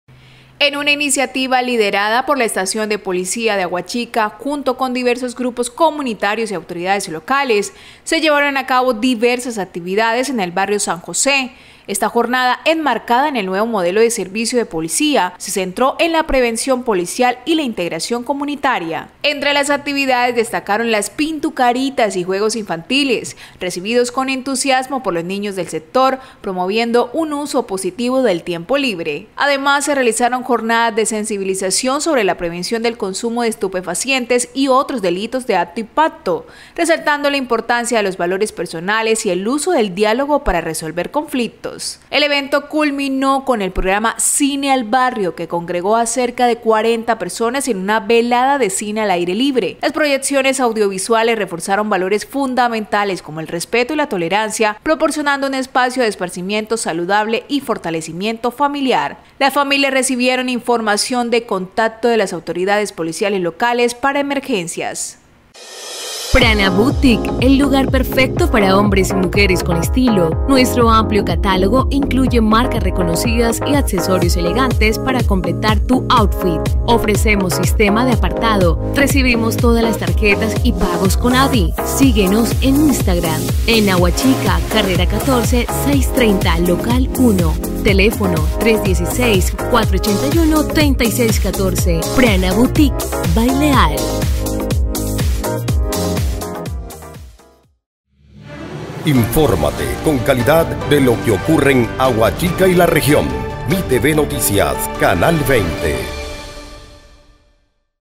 Policía y comunidad se unen en jornada de integración y prevención
La Policía de Aguachica realizó una exitosa jornada comunitaria en el barrio San José, combinando actividades lúdicas, sensibilización y cine al aire libre para fortalecer lazos con la comunidad y prevenir delitos.